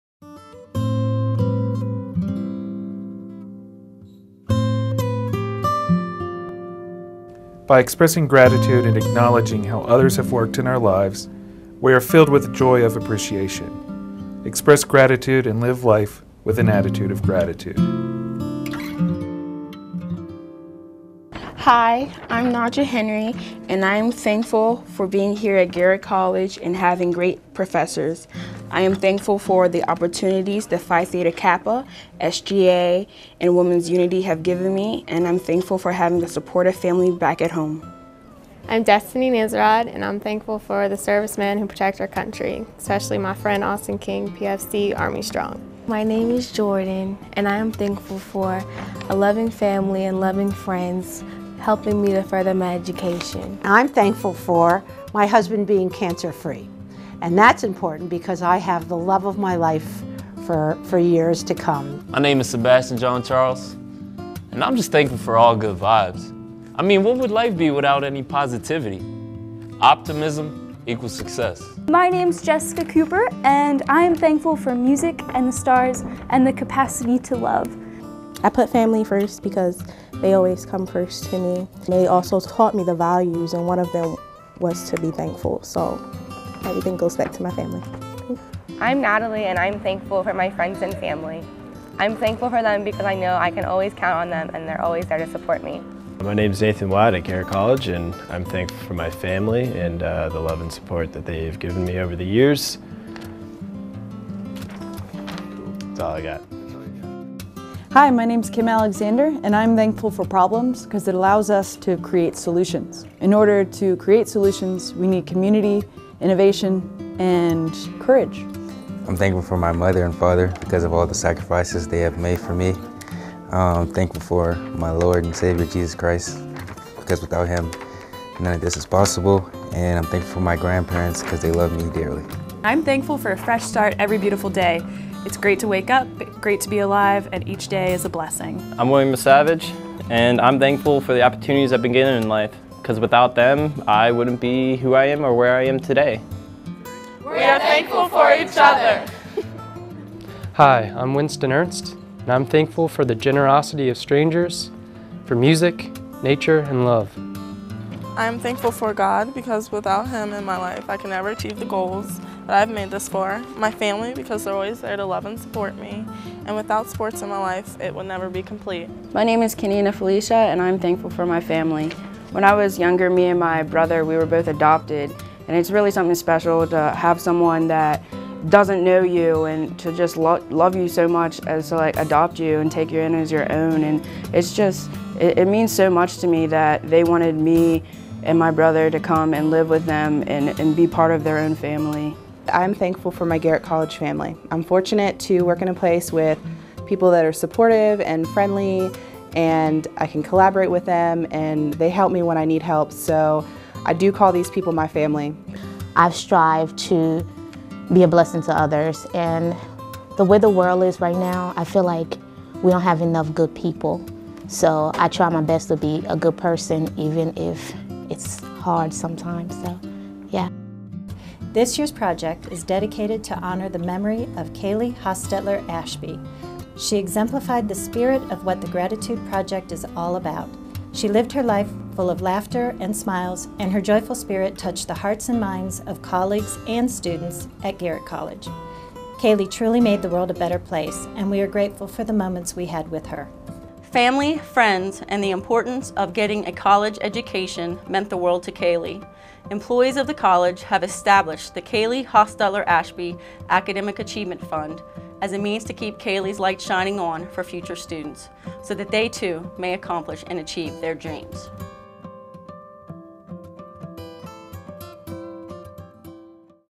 The Gratitude Project
The Gratitude Project at Garrett College is a forum where the College community recognizes and vocalizes their gratitude for others.  The 2015 Gratitude Project is dedicated to honor the memory of Kalie Ashby, a native of Oakland, Md. and Garrett College alumnus and staff member. Kalie exemplified the spirit of what the Gratitude Project is all about. Her joyful spirit touched the hearts and minds of colleagues and students and the College community is grateful for the moments we had with her.

Employees of the College recently established the Kalie Hostutler Ashby Academic Achievement Fund. Kalie believed in the future and possibilities that a higher education could bring to today’s young people. She was a vocal advocate for the high school dual enrollment program with the College and encouraged local students to make the most of the opportunities available to them. Through this fund, we hope to encourage and inspire Garrett College students to have the desire to pursue the highest level of academic achievement possible, just as Kalie did.

For more information on making a gift to the Kalie Hostutler Ashby Academic Achievement Fund, contact the Garrett College Foundation at . All gifts made through the Foundation are tax deductible to the fullest extent of the law.